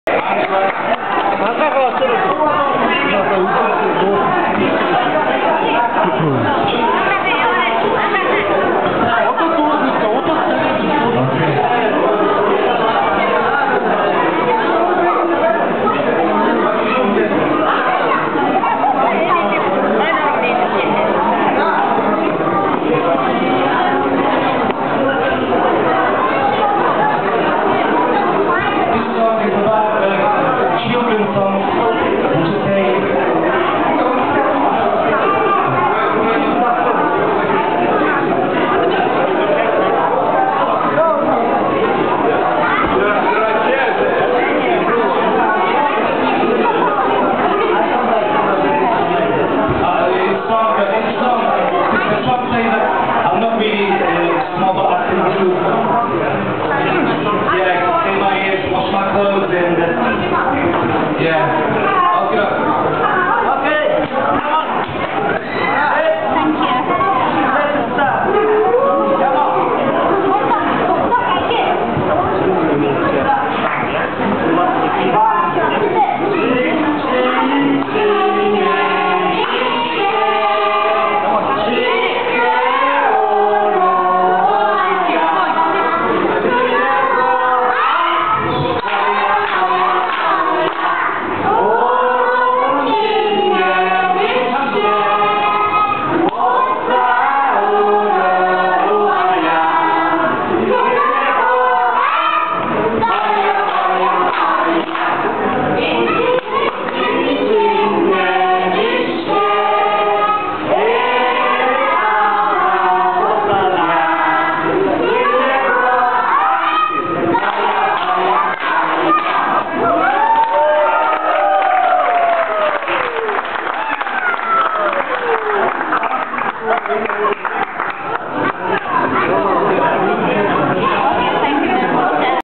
Mongolian Night 2011 @ NNRF - #4 children's choir
The Small World Cafe is a series of evenings exploring the culture of some of Nottingham's immigrant communities, hosted at the Nottingham and Nottinghamshire Refugee Forum (NNRF). These videos are from the Mongolian Night of 2011 - a wonderful evening of a film, displays, music, singing, dancing, and national costume.

James and Julia Howell lead a hastily assembled group of Mongolian children in singing a Mongolian song.